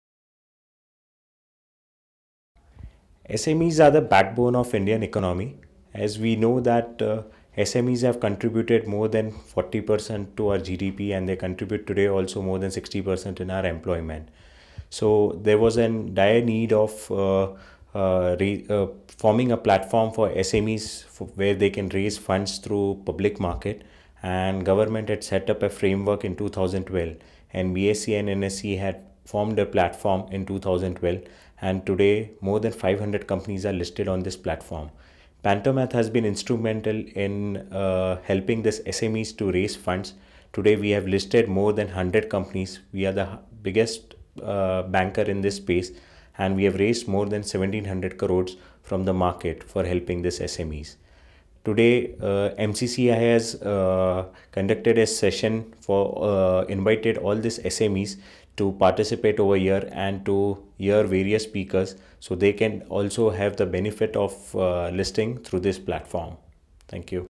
Unconventional Ways of Raising Finance for MSMEs
Speaker : Mr. Saahil Kinkhabwala, Dy. VP, Pantomath Advisory Services Pvt Ltd
Saahil speaks about procedures and methods followed by listing, valuation of business, eligibility criteria, ROC compliance, for listing, at our event on unconventional ways of raising finance for MSMEs